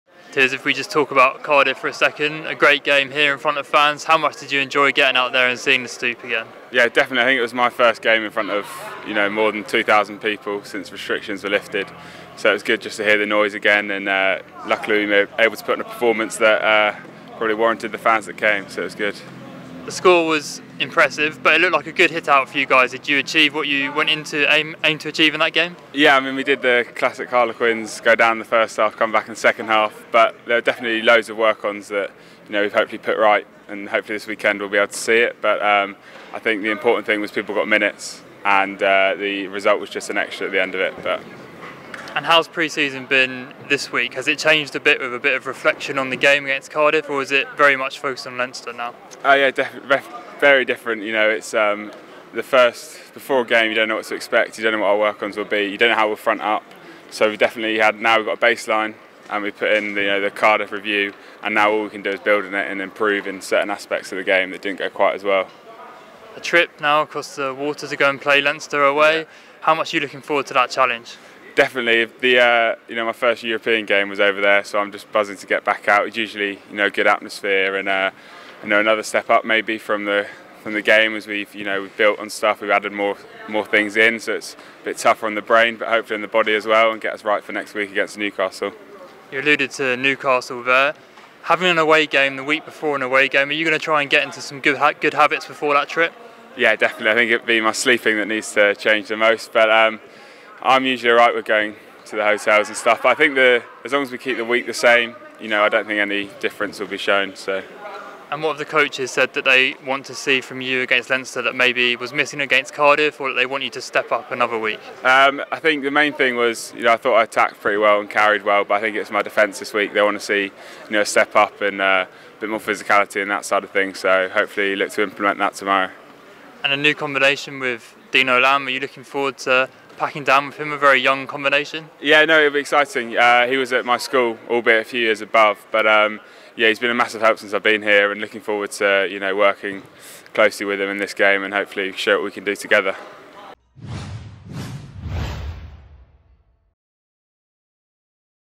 Hugh Tizard excited by Leinster challenge
The Harlequins lock says the Cardiff game was a good exercise to give ourselves a baseline but now we must improve against Leinster.